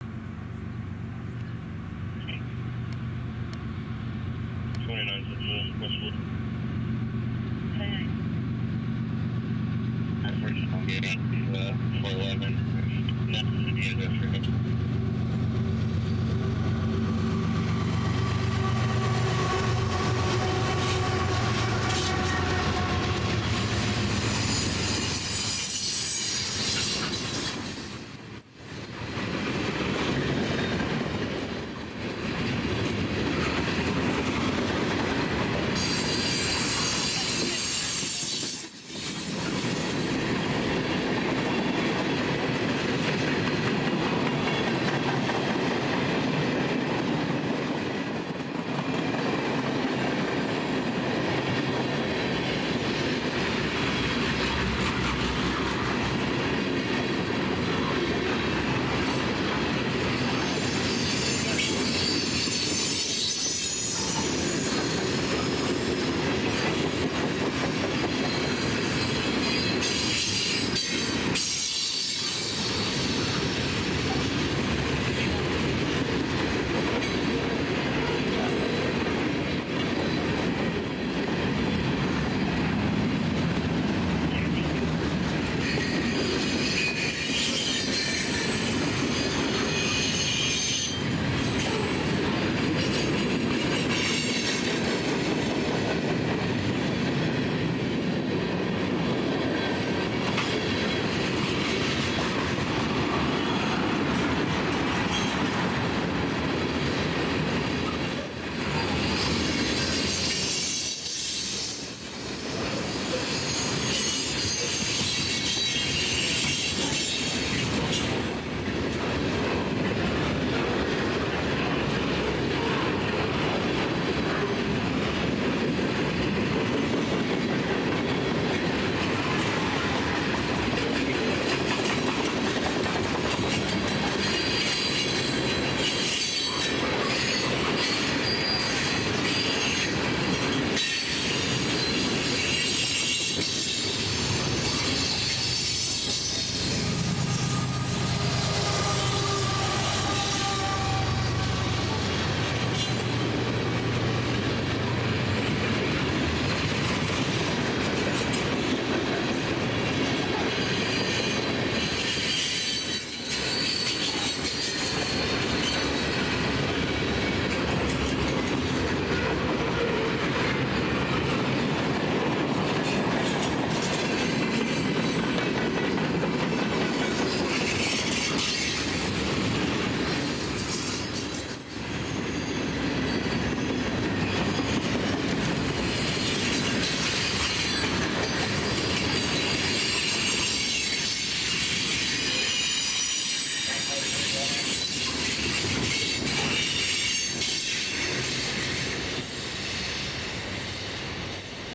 CN M302
1/6/24
302 rolls by with 550 axles of train with 2 CNs leading the way and another the mid train dpu